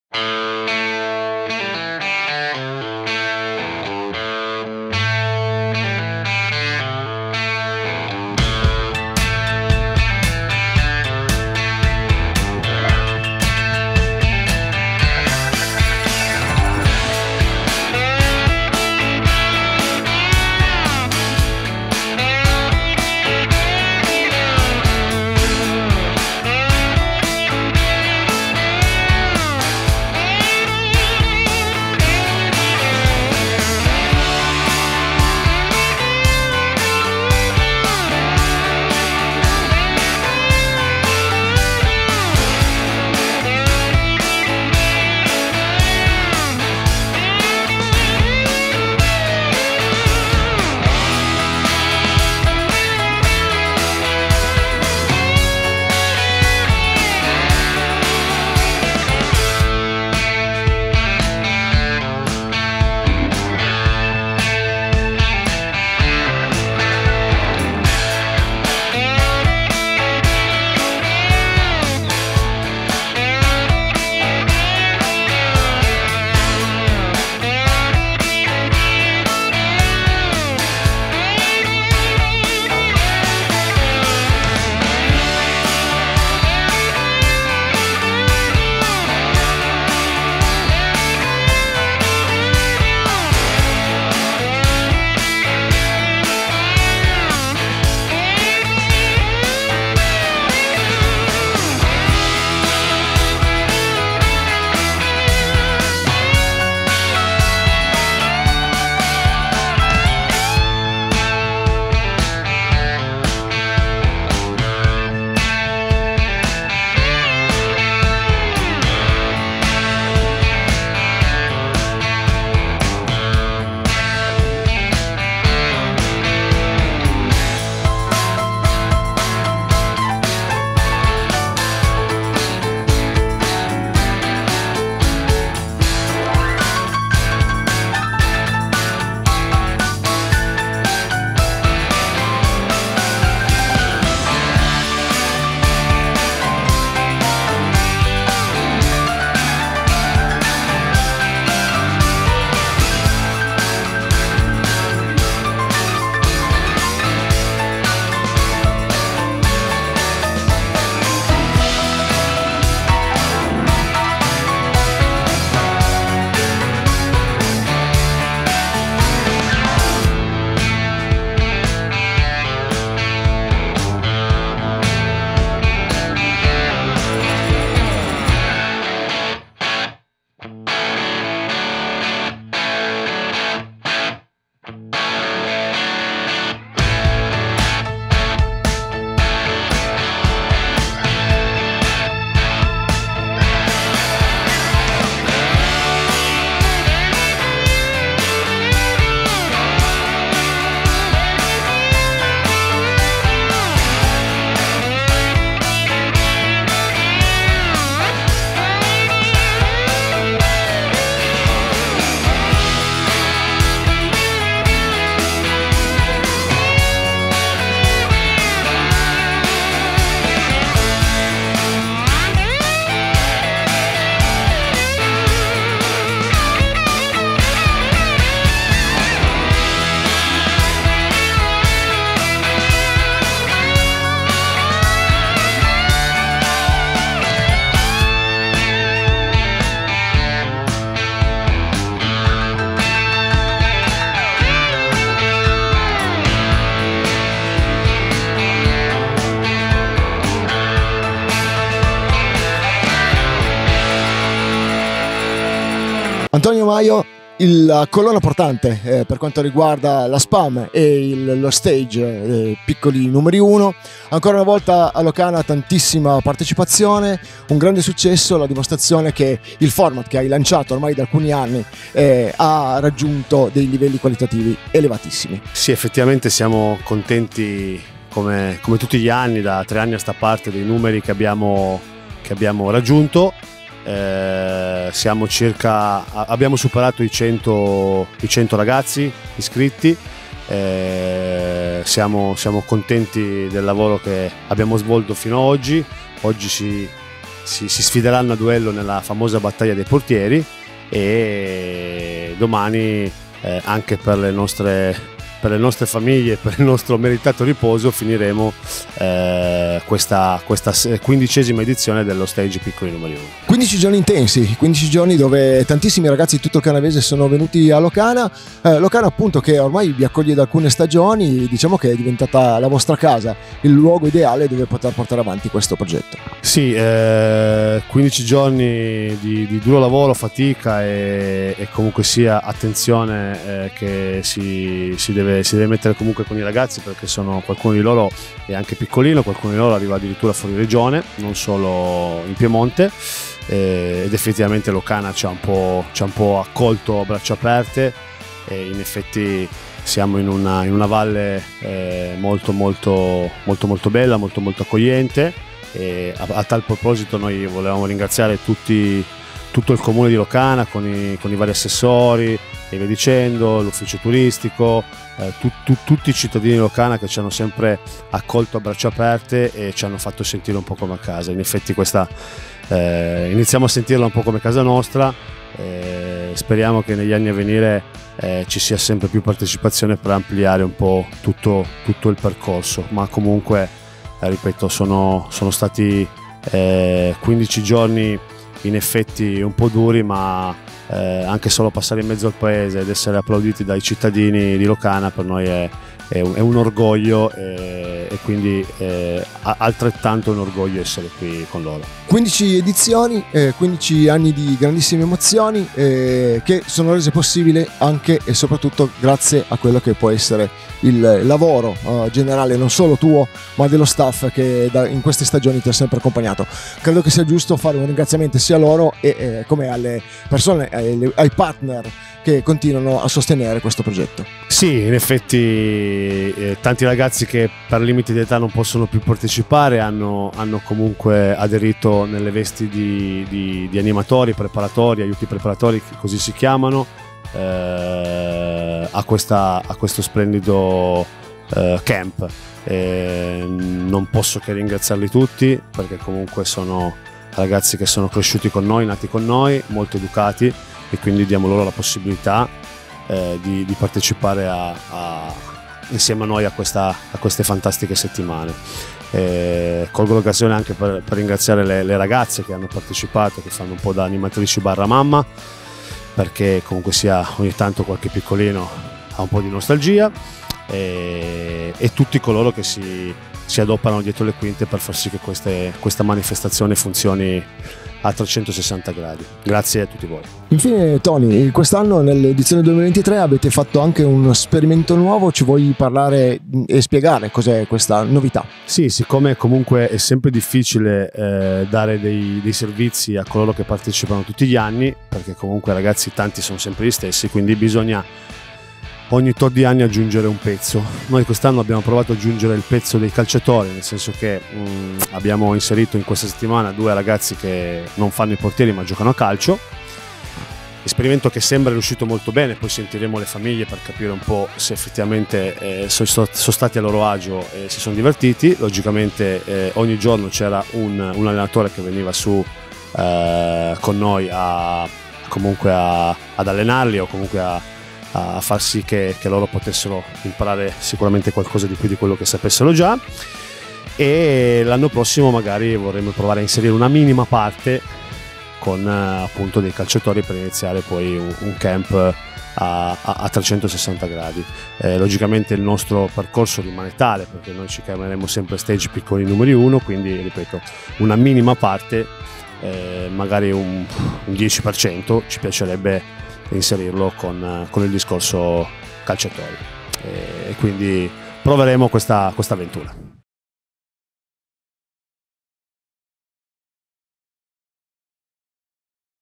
LOCANA - I piccoli numeri 1 non deludono
Ancora una volta il bilancio finale è di quelli eccezionali, con numeri di tutto rispetto, per 15 giorni dove sport, amicizia e voglia di stare insieme. La quindicesima edizione dello stage “Piccoli numeri 1”, organizzato dalla Spam, ovvero l'associazione sportiva guidata da Antonio “Tony” Maio va in archivio dopo due settimane nelle quali sono stati circa 110 i portieri, di tutte le età, che hanno lavorato, divertendosi, sul campo, ma hanno anche avuto modo di fare amicizia, vivere belle gite ed avventure che senza dubbio hanno fatto (e faranno) bene al cuore ed alla mente di questi atleti.

Il link dell'articolo sul nostro giornale